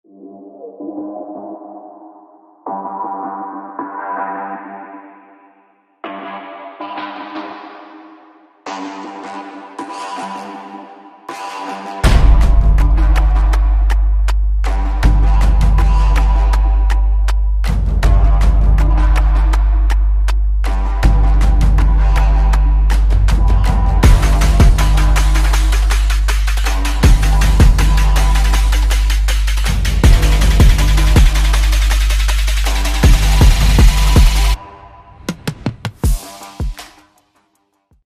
Timelapse Road - Rincon Osa to Drake Bay
On my recent trip to Costa Rica one safe alternative road to take if you want to visit Drake Bay and Drop in point to Corcovado National Park